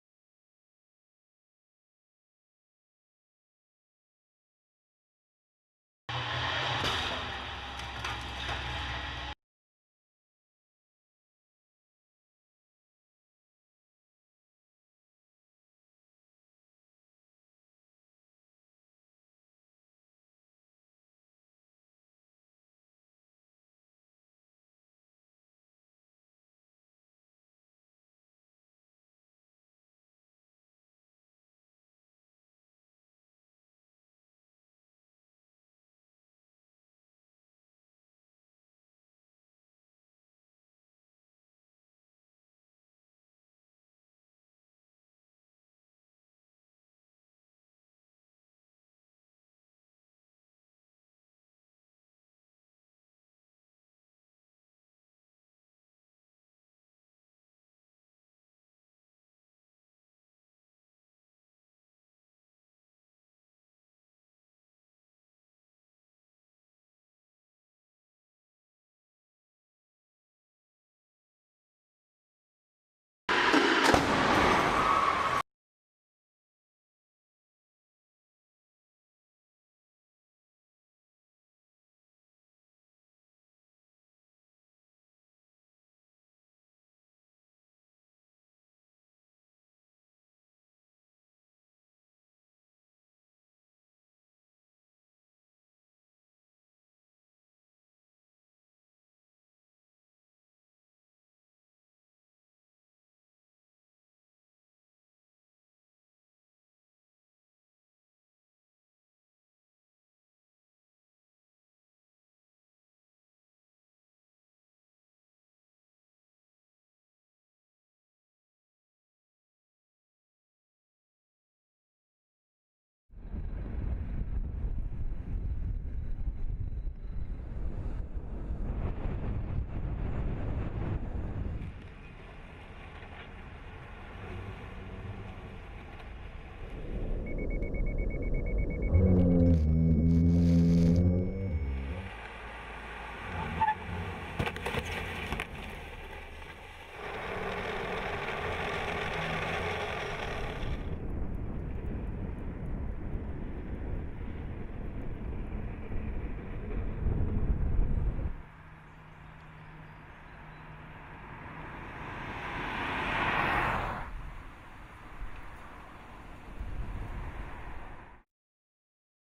Crash & Safety Tests of Subaru Forester 2019 Best in Class Small Off Road MPV
BMW
MERCEDES
AUDİ
FERRARİ